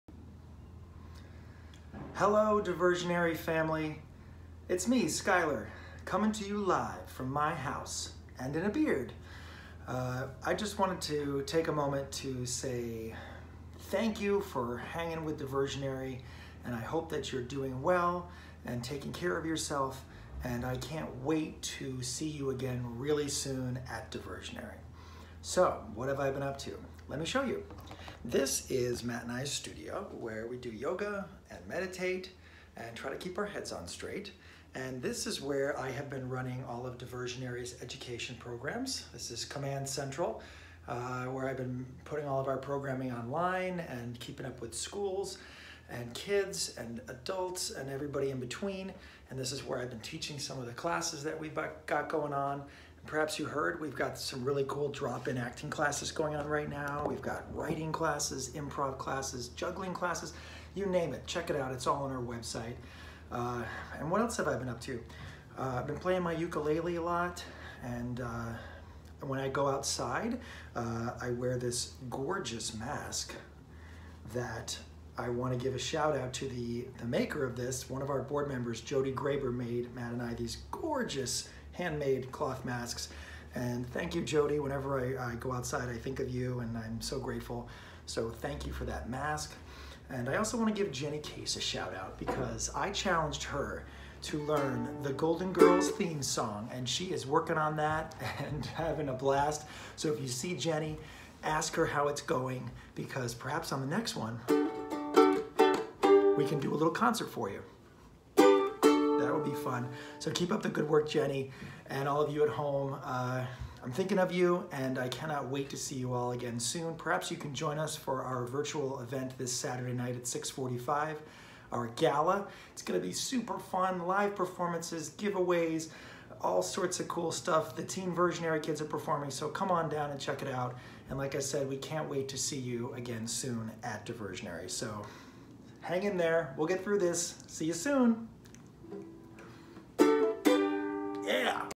Quarantine Update: Skyler Live! In a Beard!
Another Diversionary Team update from Quarantine! Hear all about what Diversionary's Director of Arts Education and Outreach has been up to, and what he's doing to keep his head on straight. Learn all about the cool classes the education department has been putting together for YOU to enjoy